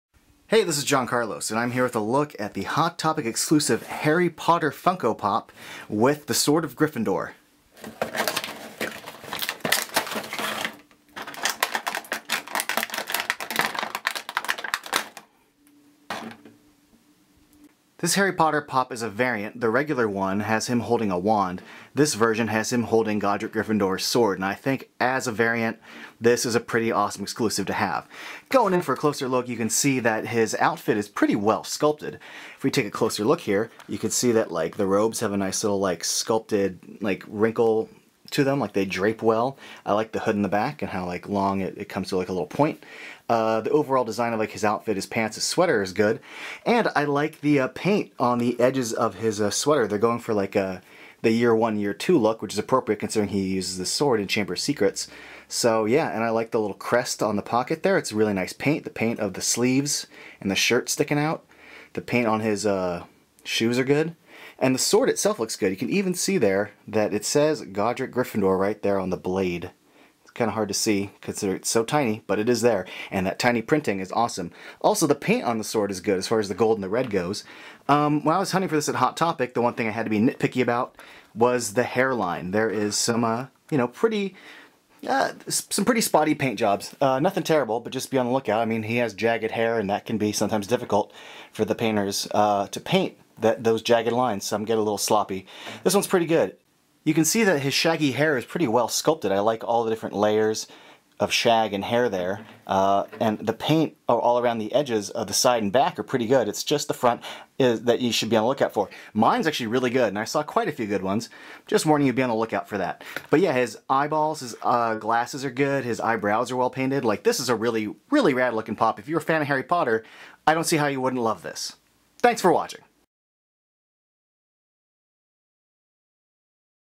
HARRY POTTER with SWORD Funko Pop review
Unboxing & review of Hot Topic exclusive Harry Potter with Sword of Godric Griffindor Funko Pop! vinyl figure. by John Carlos McMaster.

toy
variant